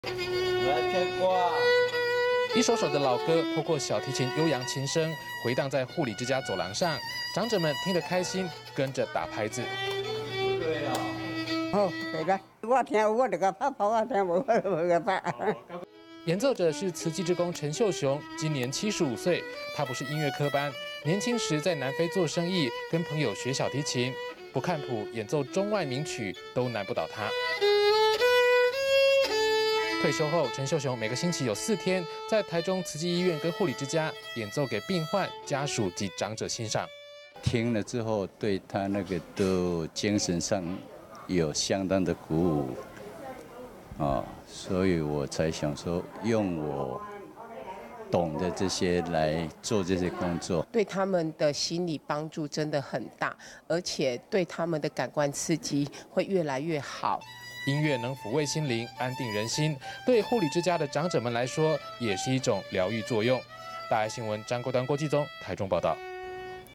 以音樂會友 中慈護理之家飄提聲
在醫療之外，一分關懷，一首樂曲，也可以是一貼藥，膚慰病苦。在台中慈濟醫院，一周有四天，這位小提琴樂手，會進駐，演奏中外名曲，讓醫院，有一方音樂殿堂。而演奏者陳秀雄，已75歲了，以服務病患為樂。

一首首的老歌，透過小提琴悠揚琴聲，迴盪在護理之家走廊上，長者們聽得開心，跟著打拍子。

台中慈濟護理之家住民：「好 不錯，我聽得懂 我就給他打拍子，聽不懂 我就沒有打。」

演奏者是慈濟志工陳秀雄，今年75歲，他不是音樂科班，年輕時在南非做生意，跟朋友學小提琴，不看譜演奏中外名曲，都難不倒他。

退休後，陳秀雄每個星期有四天，在台中慈濟醫院跟護理之家，演奏給病患、家屬及長者欣賞。

慈濟志工 陳秀雄：「聽了之後，對他精神上有相當的鼓舞，所以我才想說 用我懂的這些，來做這些工作。」

台中慈濟護理之家護理長 梁玉欣：「對他們的心理幫助真的很大，而且對他們的感官刺激，會越來越好。」

音樂能撫慰心靈，安定人心，對護理之家的長者們來說，也是一種療癒作用。

張桂端 郭繼宗 台中報導